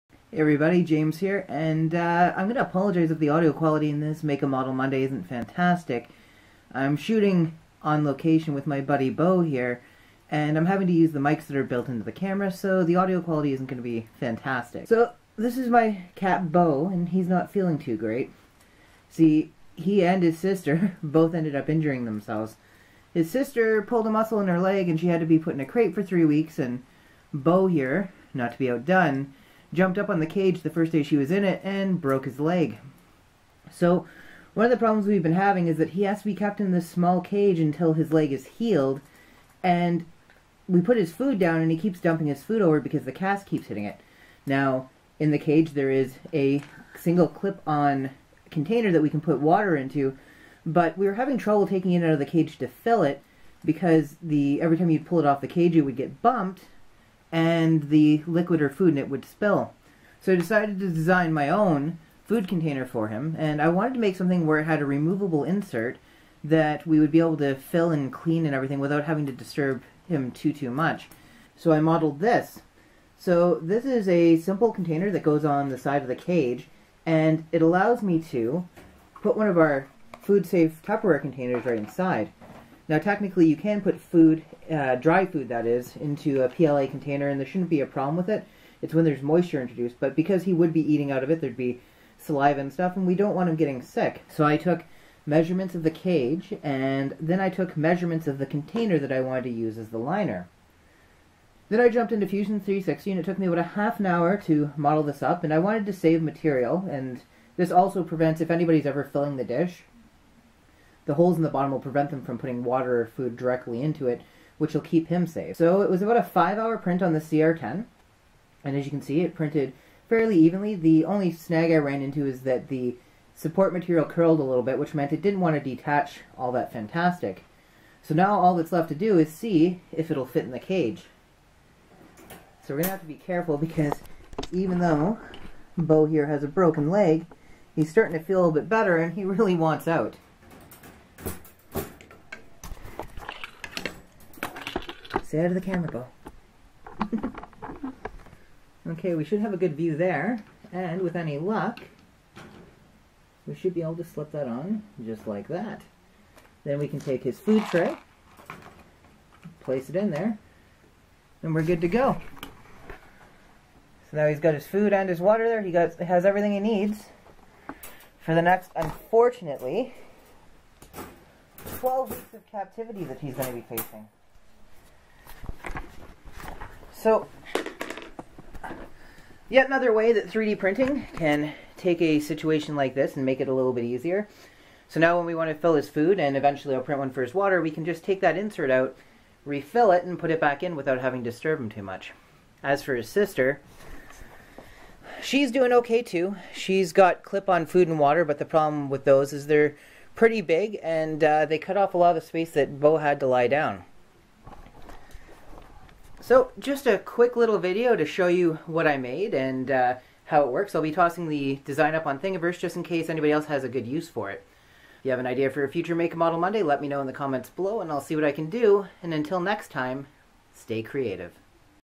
Make a Model Monday - Cage Bowls For Bowser
My little orange cat had a bit of a mishap and broke his leg. I decided to use 3D printing to, hopefully, make his cage life a little better.

If you happen to need a copy of this bowl, feel free to snag it here